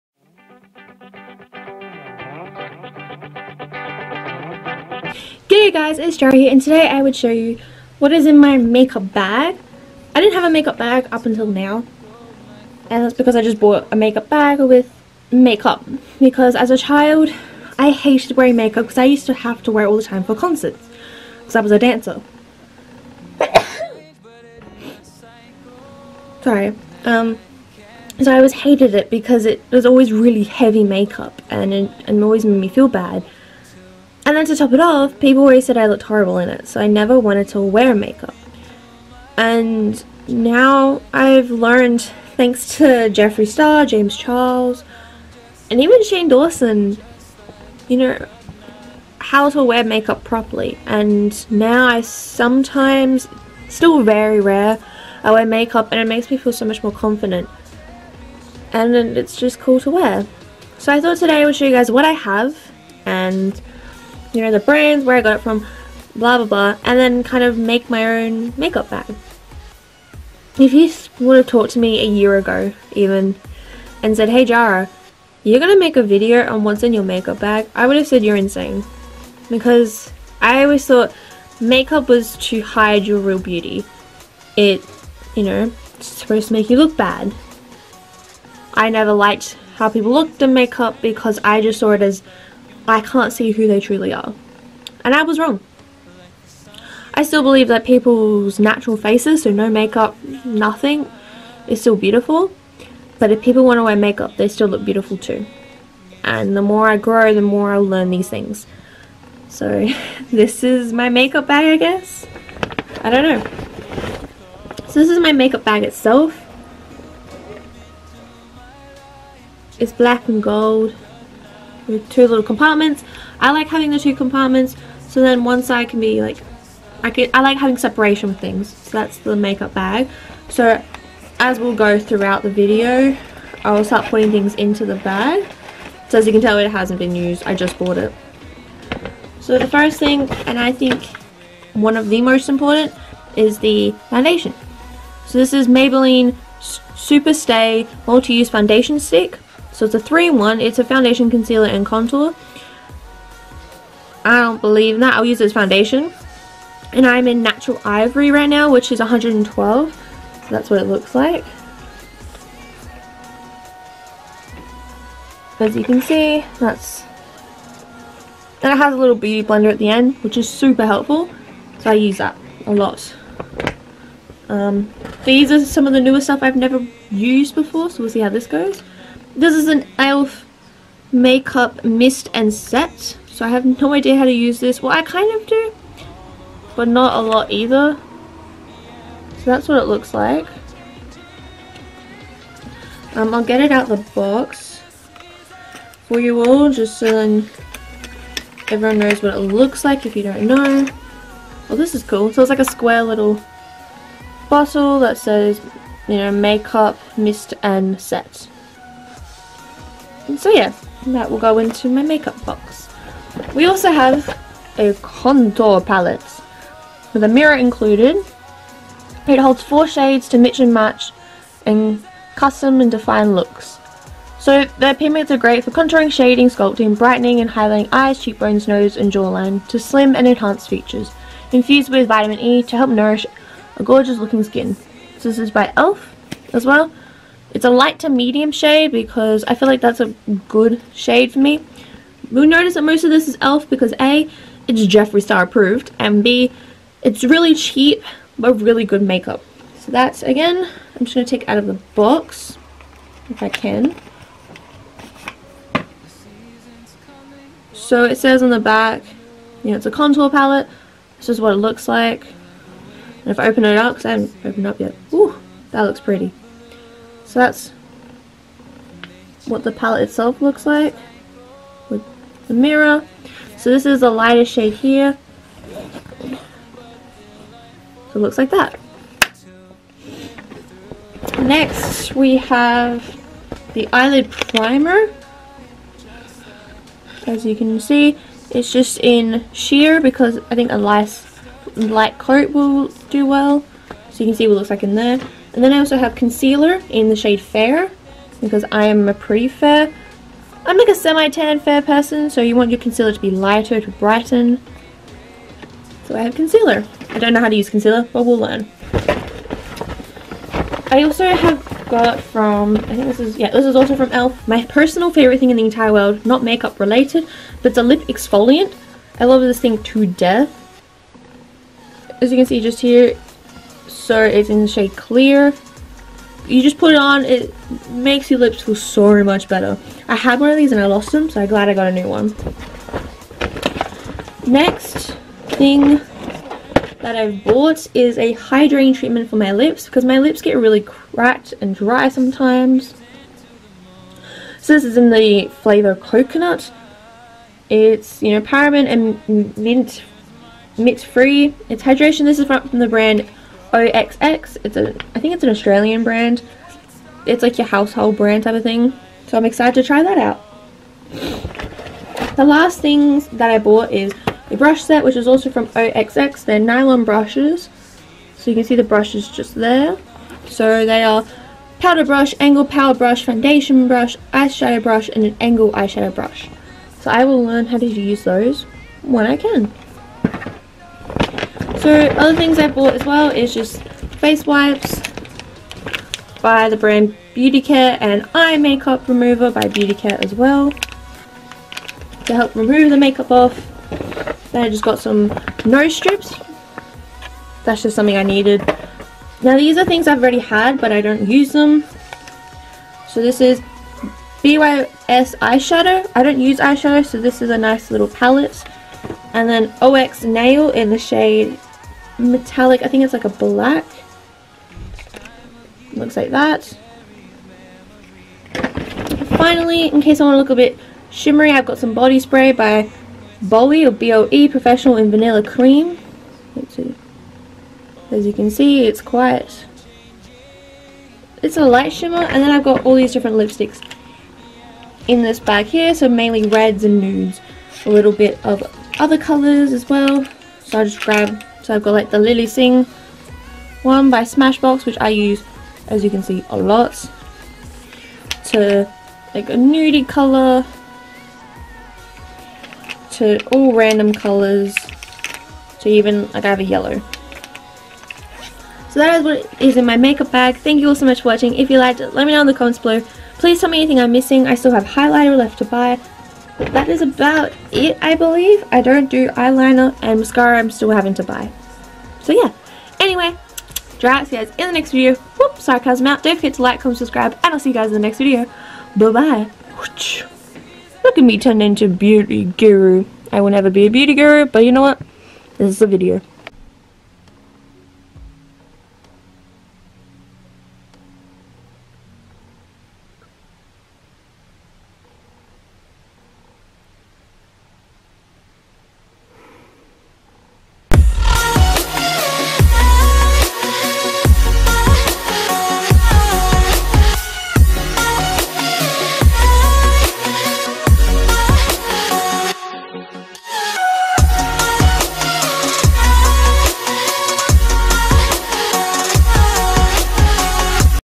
What is in My Bag - Makeup Edition 💄 💼
So this is what is in my first ever make-up bag for me that is not a concert make-up bag.